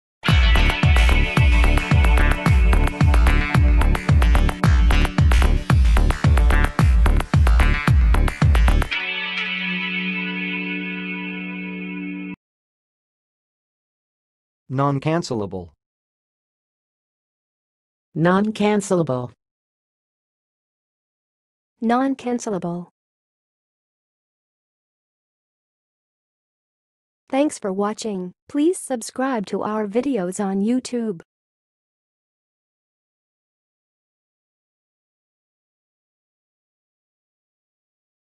How to say "noncancellable"! (High Quality Voices)
Watch in this video how to say and pronounce "noncancellable"!